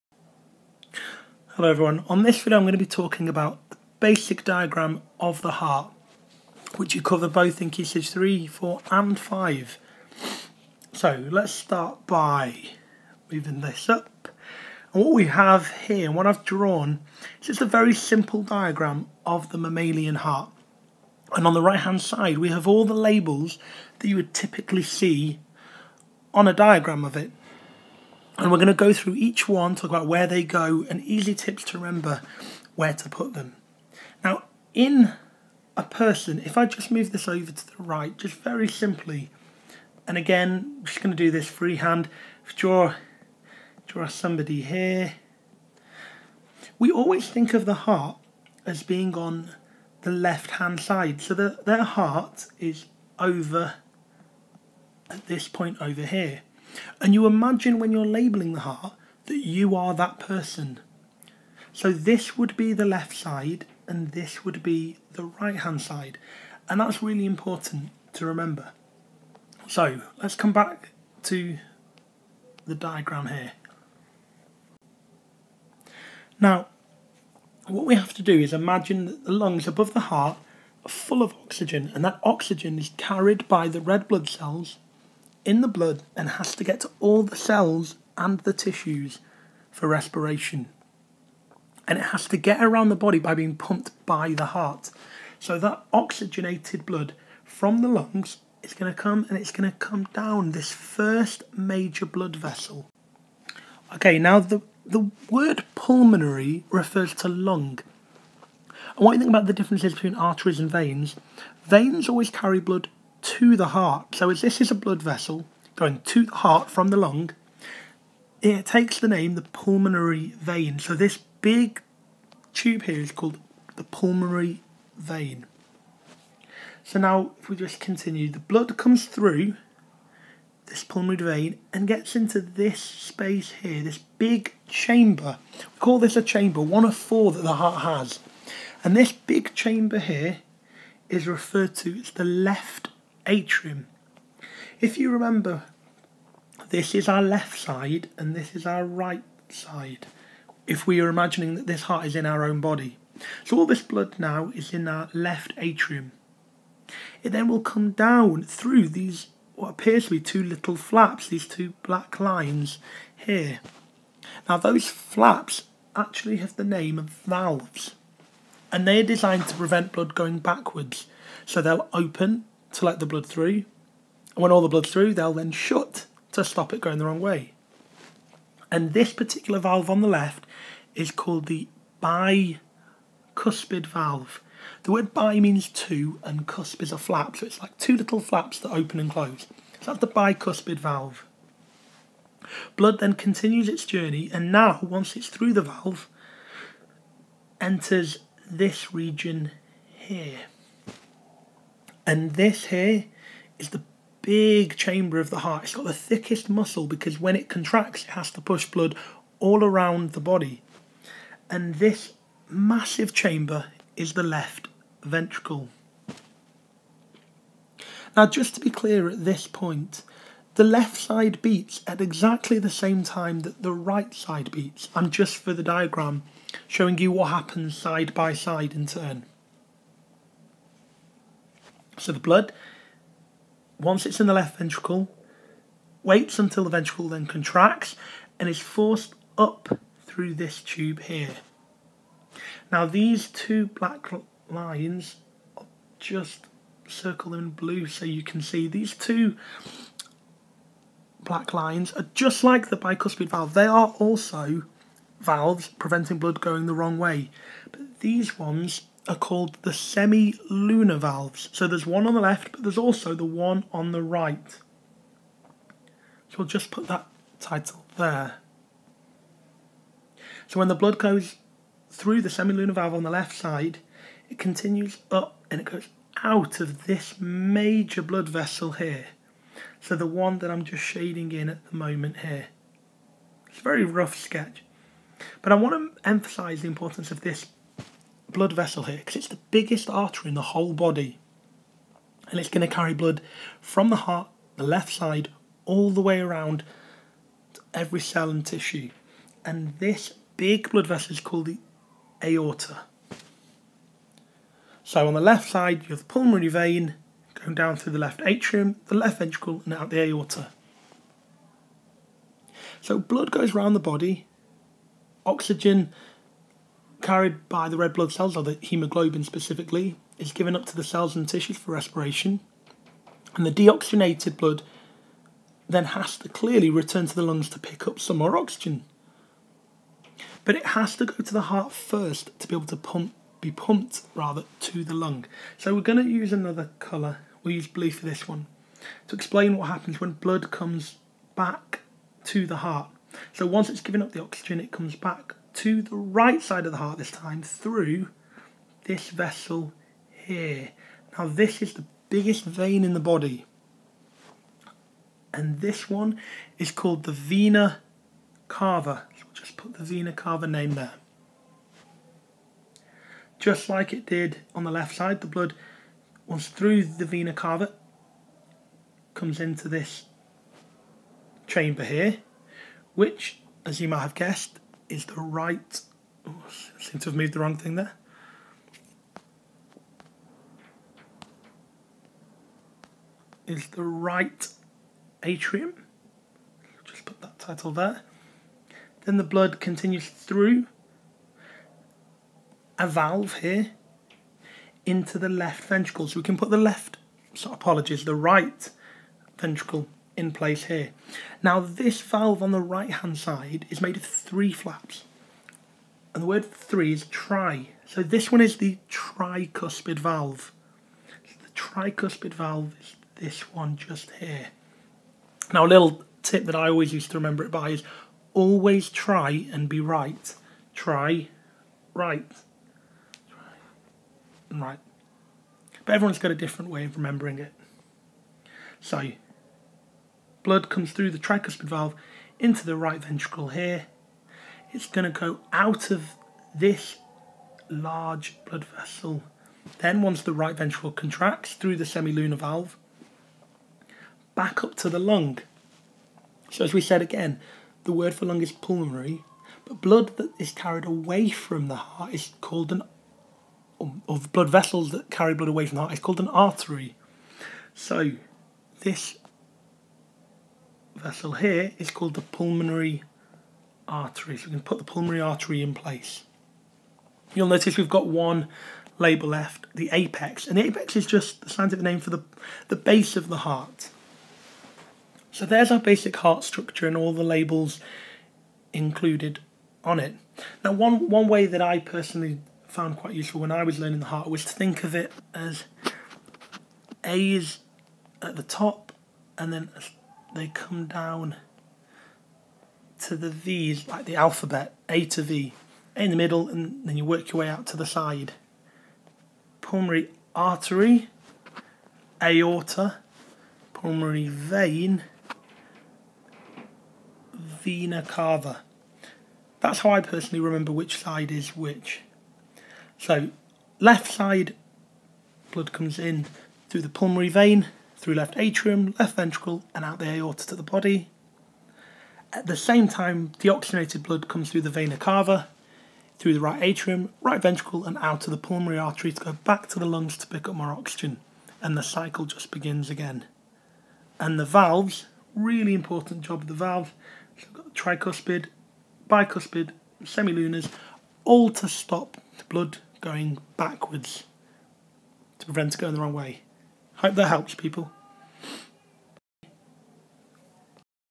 mr i explains: The Basic Structure of the Heart
In this video, I describe the gross anatomy of the heart, and give full scientific labels/names for all the key structural components. I will also give some useful tips on remembering which side is left and which is right, the names of the valves and the order of the great vessels.